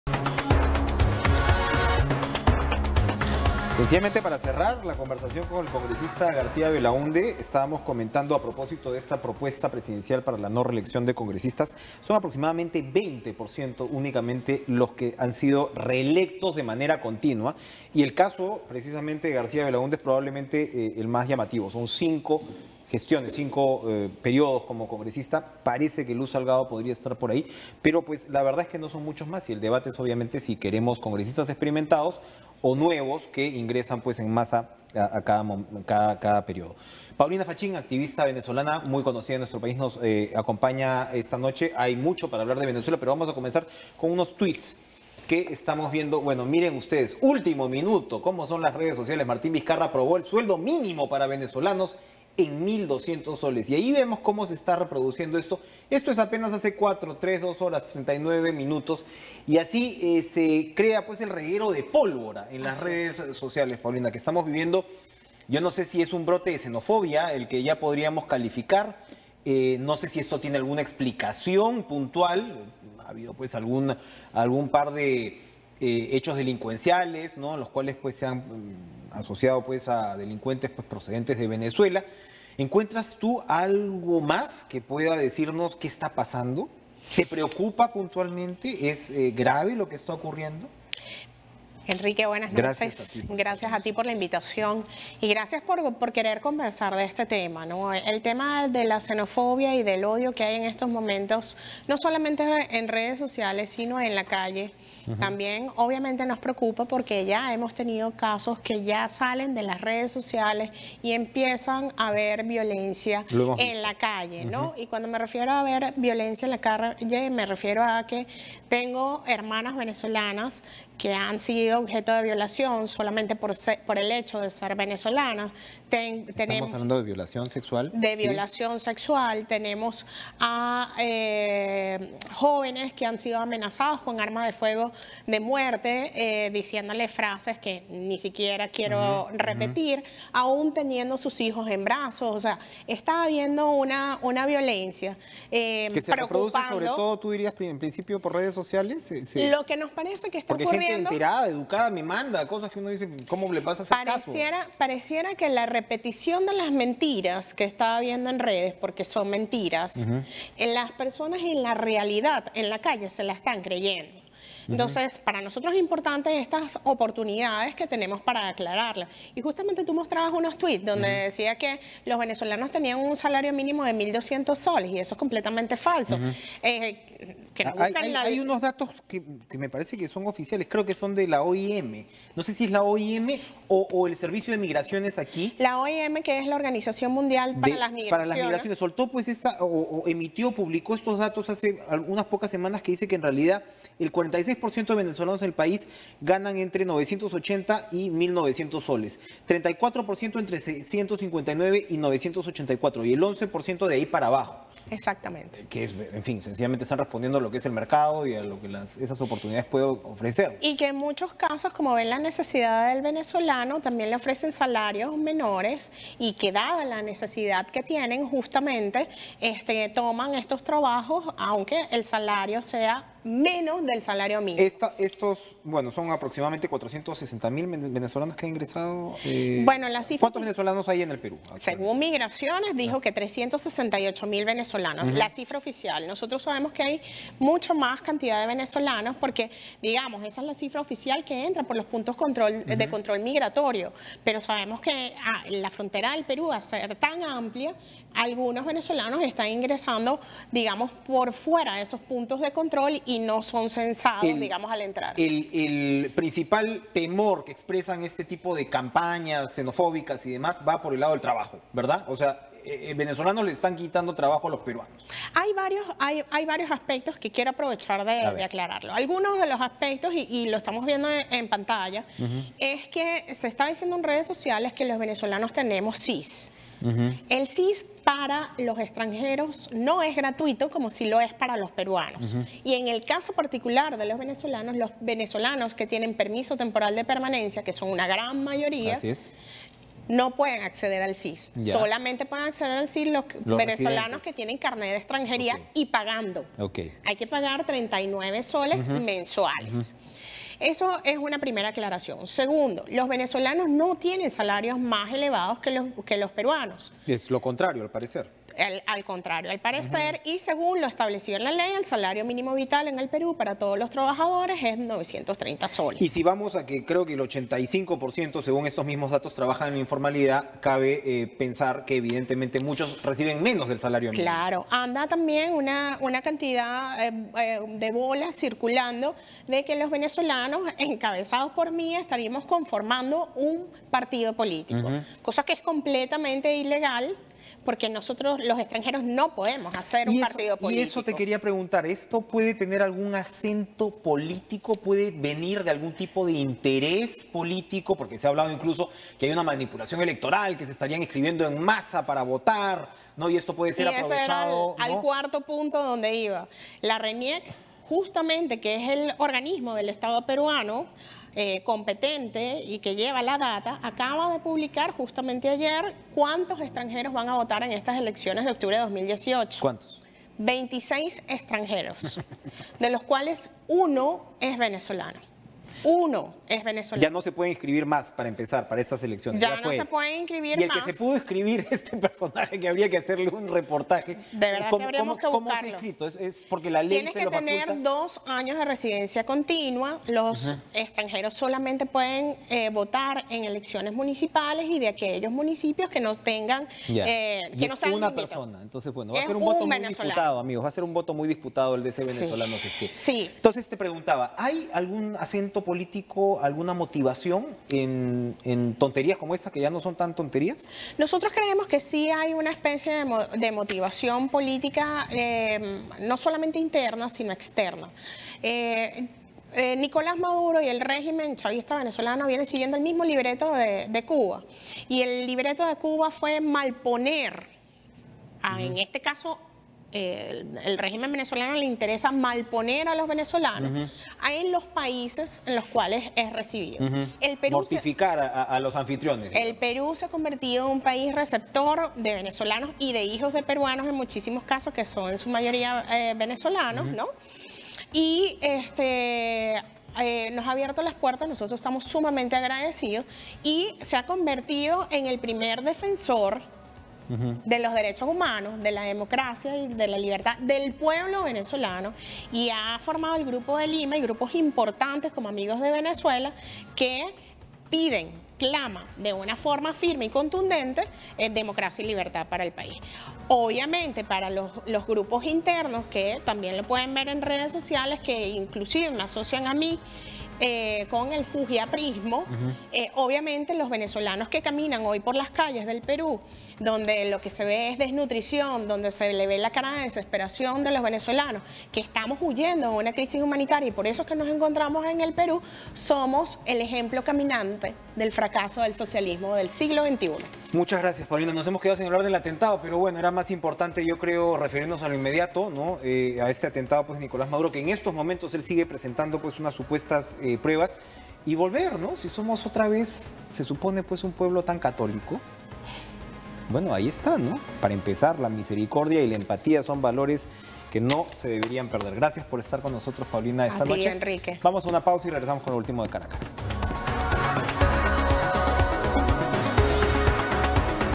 Activista venezolana denuncia campaña de desprestigio contra sus connacionales
La activista venezolana Paulina Facchin desmintió las diversas desinformaciones sobre sus compatriotas en el Perú y denunció que detrás de esta campaña de desprestigio está el chavismo.